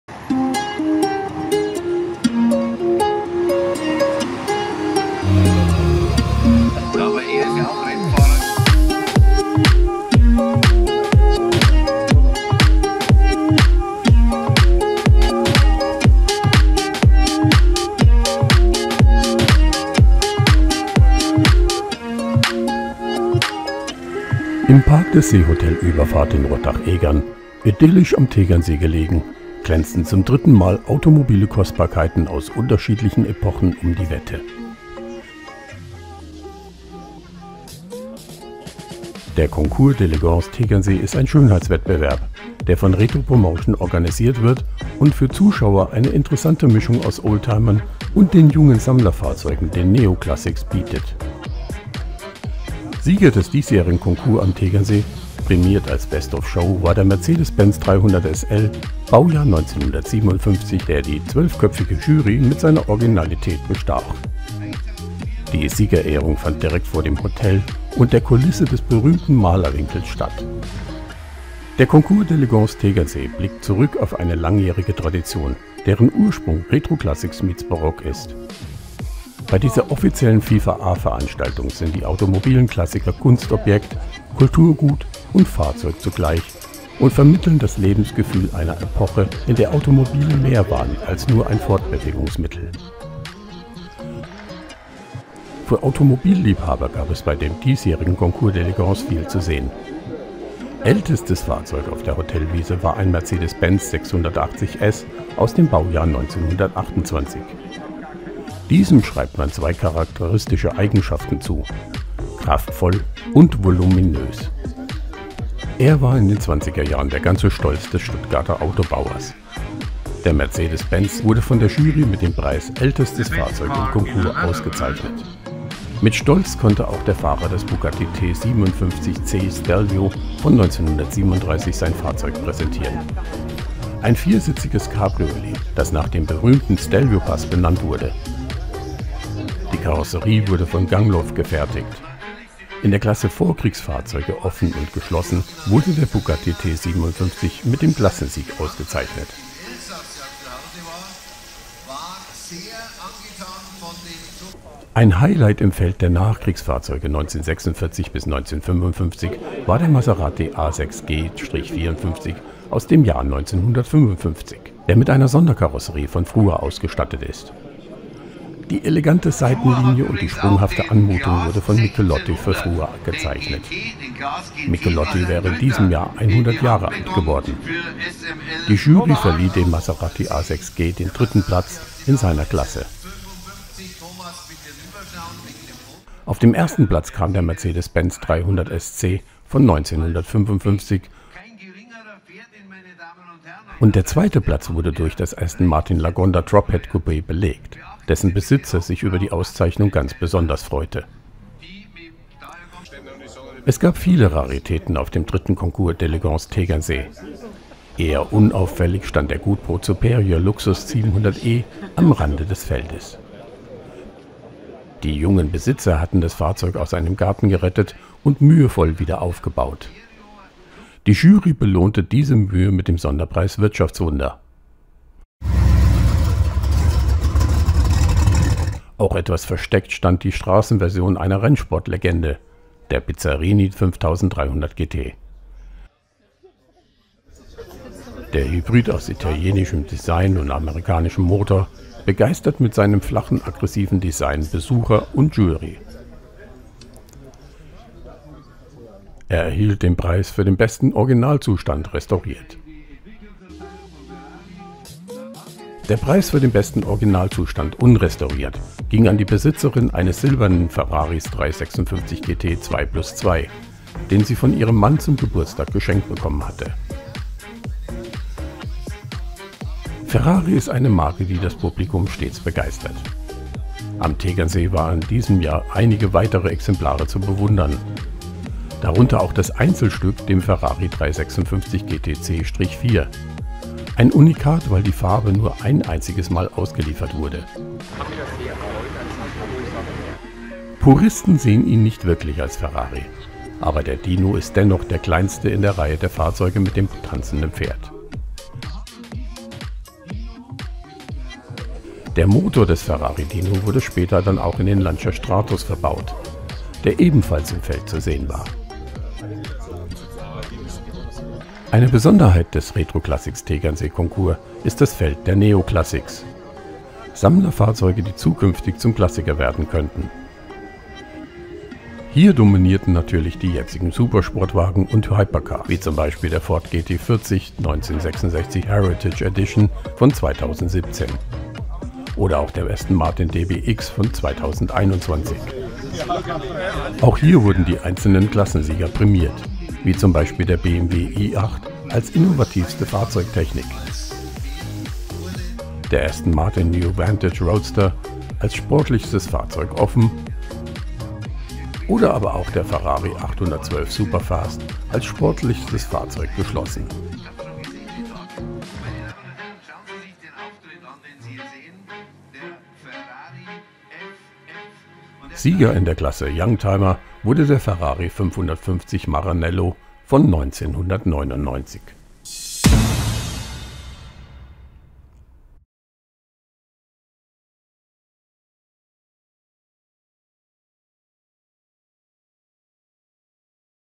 Retro Classics Tegernsee Concours D´Elegance
Im Park des Seehotel Überfahrt in Rottach-Egern, idyllisch am Tegernsee gelegen, glänzten zum dritten Mal automobile Kostbarkeiten aus unterschiedlichen Epochen um die Wette. Der Concours D´Elegance Tegernsee ist ein Schönheitswettbewerb, der von Retro Promotion organisiert wird und für Zuschauer eine interessante Mischung aus Oldtimern und den jungen Sammlerfahrzeugen, den Neo Classics bietet.
Sieger des diesjährigen Concours am Tegernsee, prämiert als „Best of Show“, war ein Mercedes-Benz 300SL Baujahr 1957, der die zwölfköpfige Jury mit seiner Originalität bestach. Die Siegerehrung fand dierkt vor dem Hotel und der Kulisse des berühmten Malerwinkels statt.
Der Concours D´Elegance Tegernsee blickt zurück auf eine langjährige Tradition, deren Ursprung „Retro Classics meets Barock“ (früher im Residenzschloss Ludwigsburg durchgeführt) ist. Bei dieser offiziellen FIVA-A-Veranstaltung sind die automobilen Klassiker Kunstobjekt, Kulturgut und Fahrzeug zugleich und vermitteln das Lebensgefühl einer Epoche, in der Automobile mehr waren als nur ein Fortbewegungsmittel.